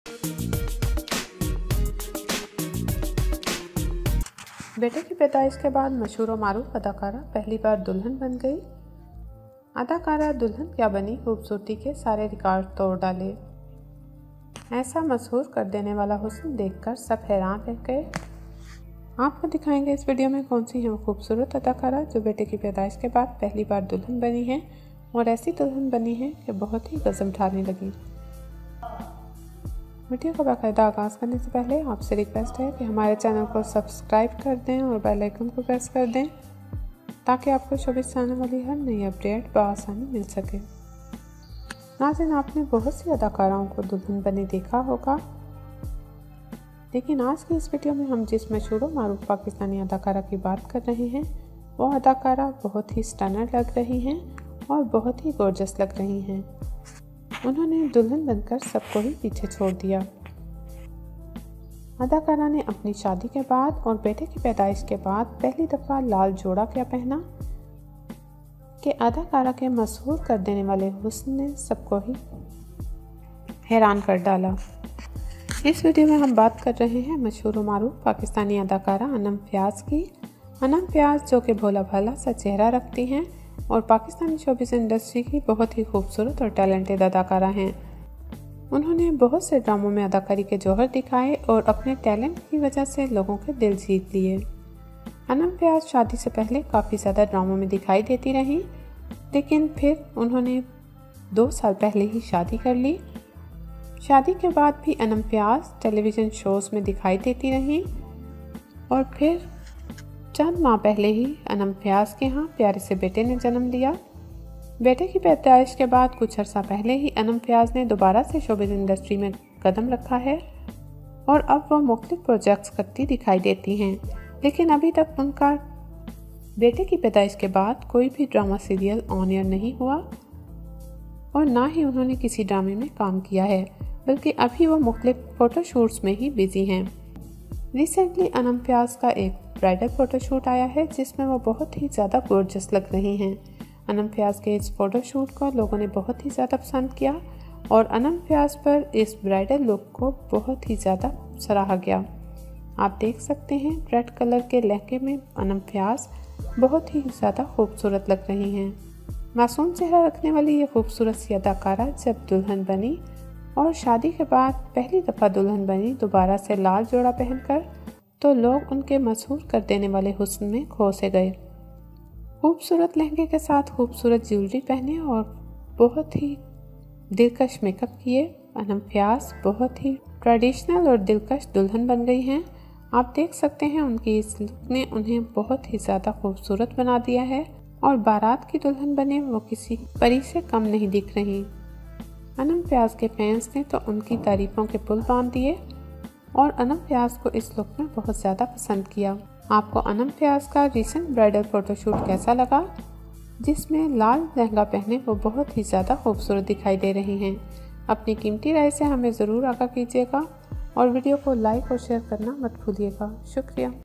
Famous Actress Became Bride after baby Birth || Mahira Khan || MK
In this video you can see Gorgeous actress in bridal look.